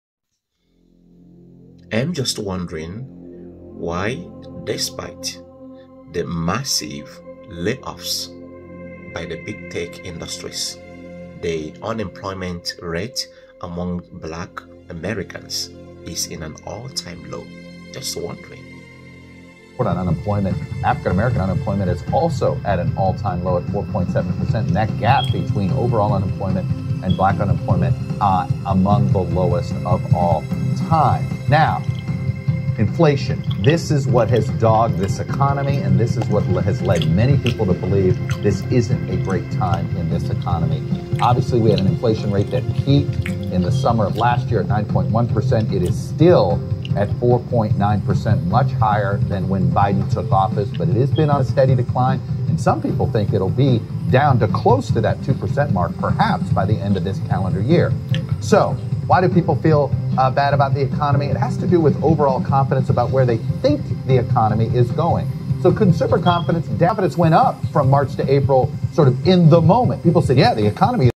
Unemployment vs Inflation. How True Is this Stat?
This video raises two questions: 1. Does it mean that the massive layoffs by big tech did not affect the black unemployment level? 2. Does it mean that Black Americans don't aren't employed in big techs? Let me know your thoughts.